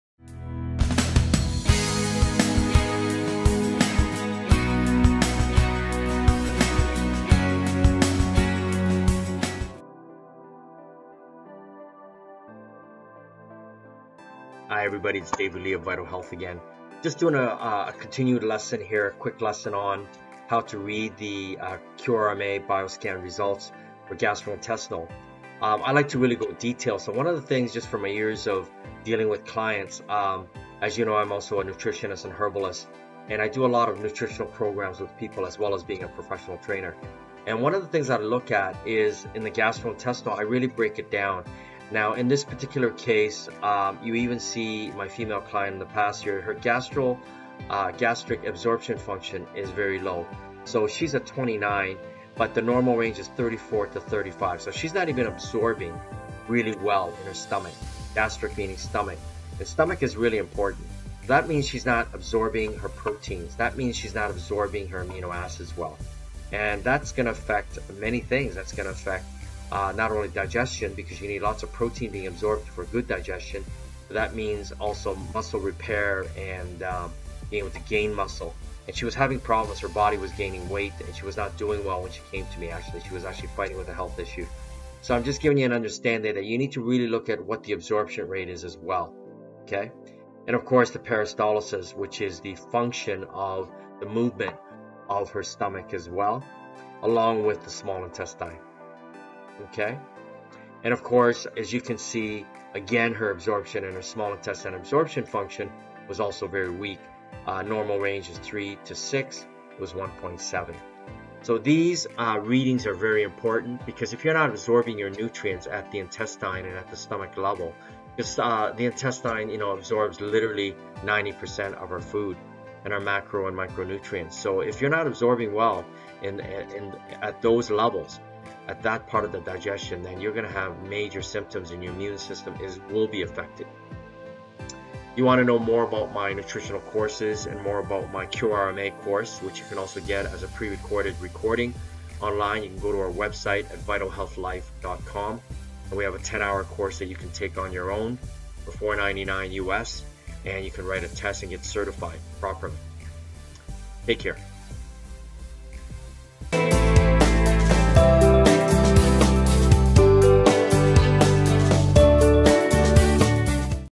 Gastro absorption
Get detailed knowledge about the Quantum Resonance Magnetic Analyser Gastro Report of a client.
Although the stomach absorbs few of the products of digestion, it can absorb many other substances, including glucose and other simple sugars, amino acids, and some fat-soluble substances. The pH of the gastric contents determines whether some substances are absorbed.